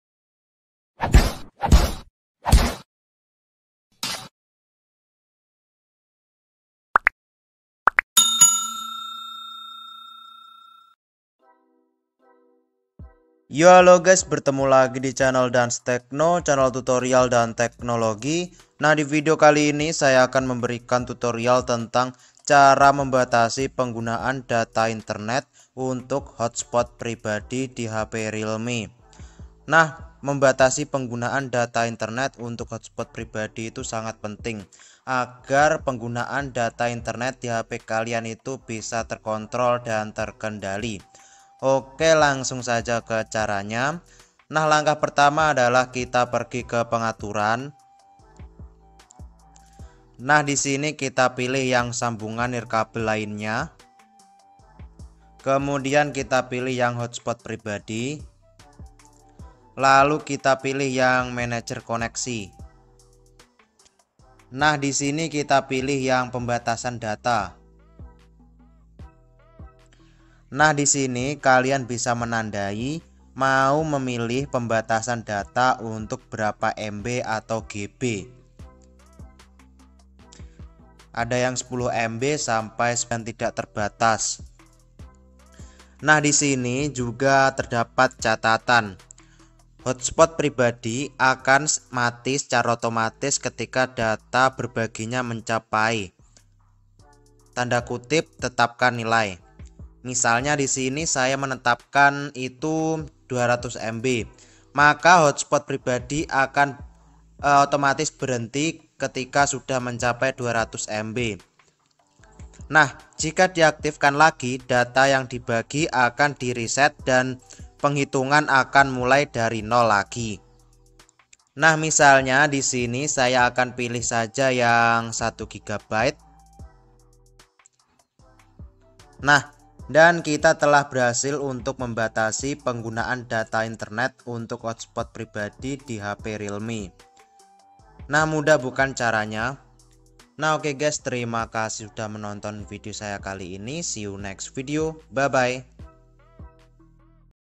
Cara Membatasi Penggunaan Data Internet Hotspot HP Realme
✨Hallo guys✨
Di video kali ini saya akan memberikan tutorial tentang Cara Membatasi Penggunaan Data Internet Hotspot HP Realme
😎 . Buat kalian yg penasaran gimana caranya, tonton video ini sampai habis. Ok👌

Happy Watching🤗

•Record: xrecorder
•Thumbnail: Pixellab
•Editing: openshot
•Background video & thumbnail: